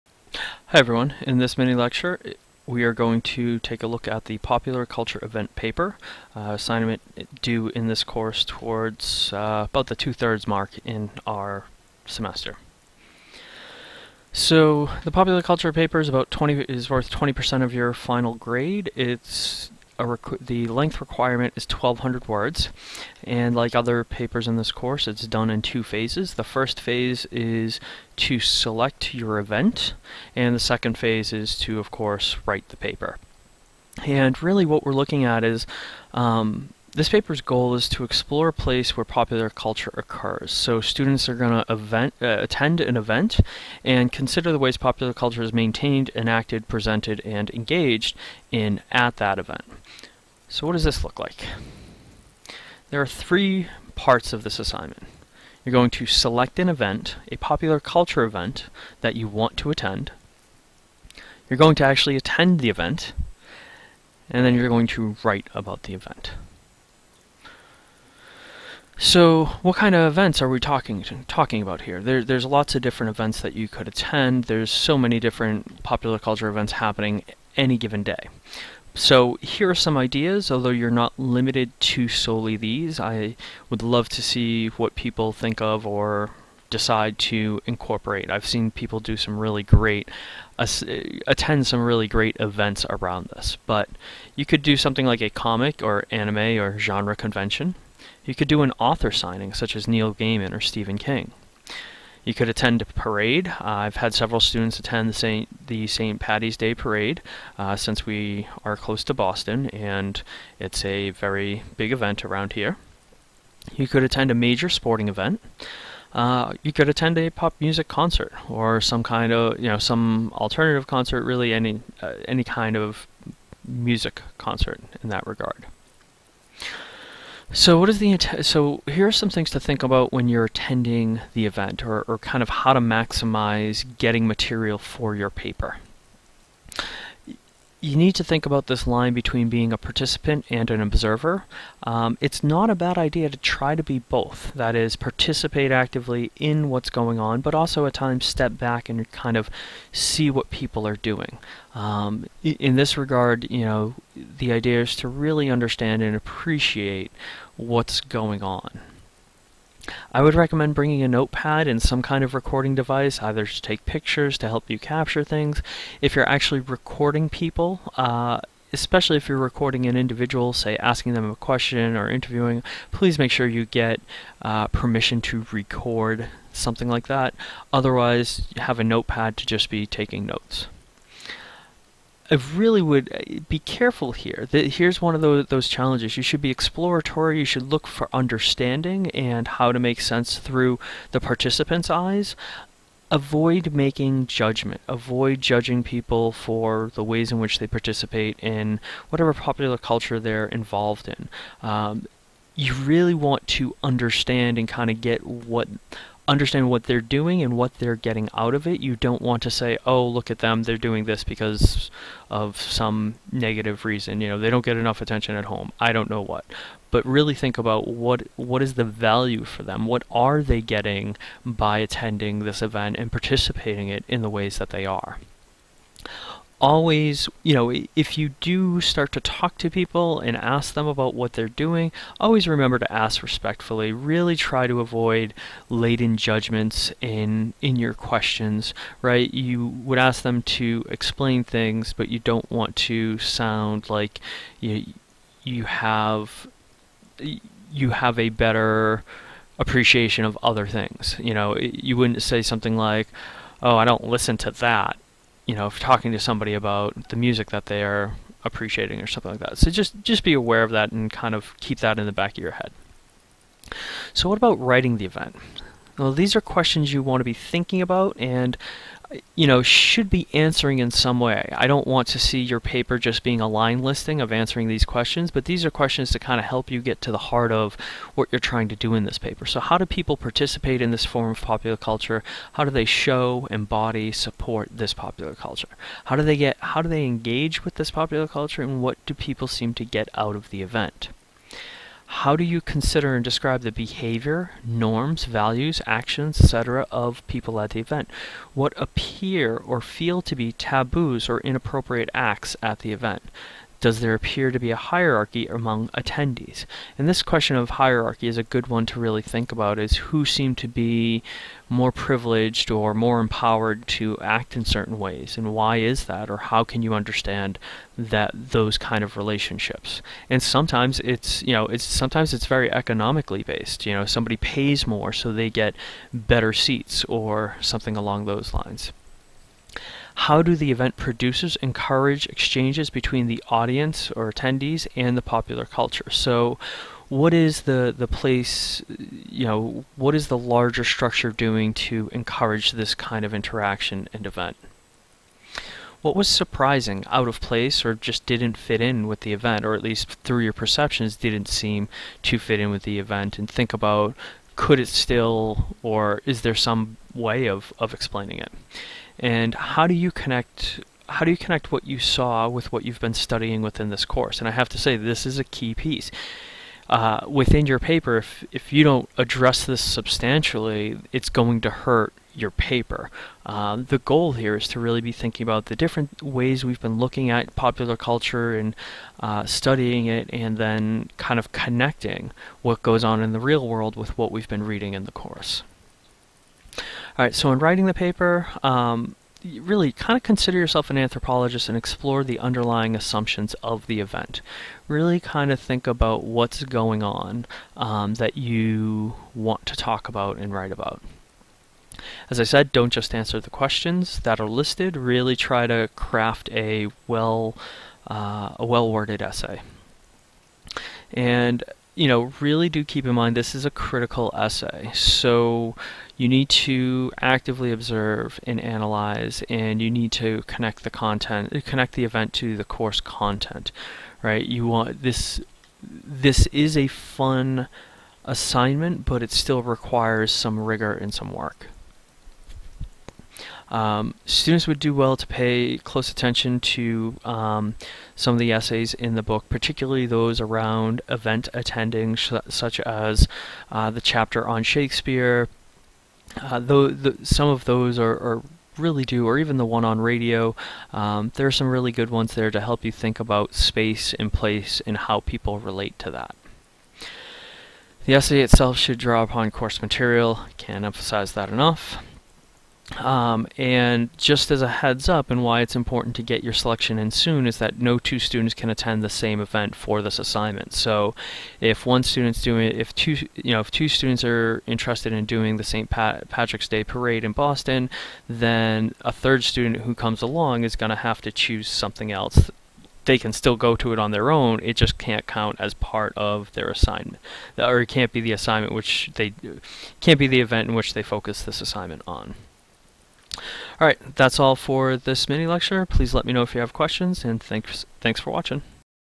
Guidelines - Popular Culture Event Paper
This is a video guideline for a Popular Culture Event paper for "Popular Culture in the U.S.", an online course at North Shore Community College.